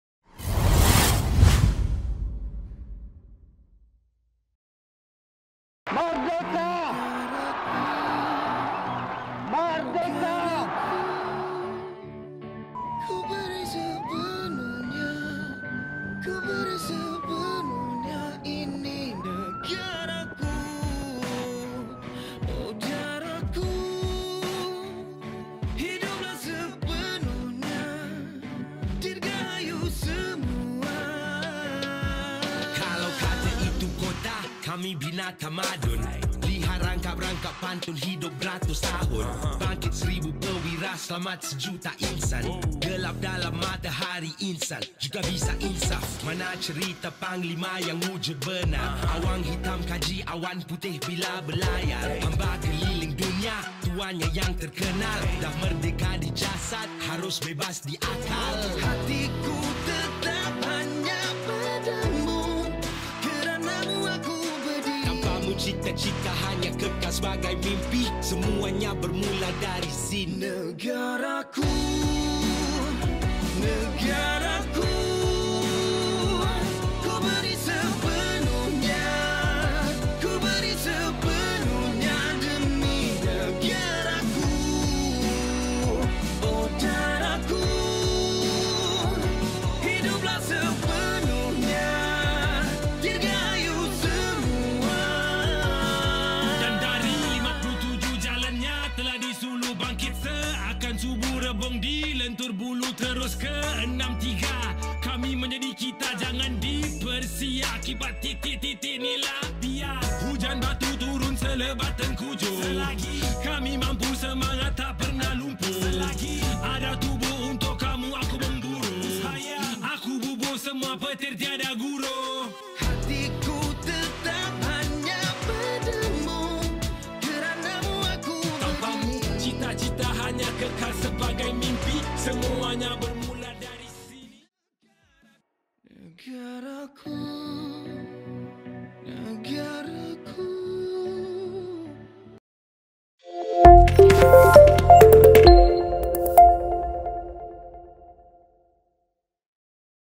Sepakat Merdeka@Komuniti Kempen Kibar Jalur Gemilang & Info On Wheels Bersepadu Daerah Kerian 2021
Liputan Video Sepakat Merdeka@Komuniti Kempen Kibar Jalur Gemilang Dan Info On Wheels Bersepadu Daerah Kerian 2021 di Dewan Dato Zainal Abidin Zain Bagan Serai dan Dewan Kerian Permai, Parit Buntar pada 27 OGOS 2021.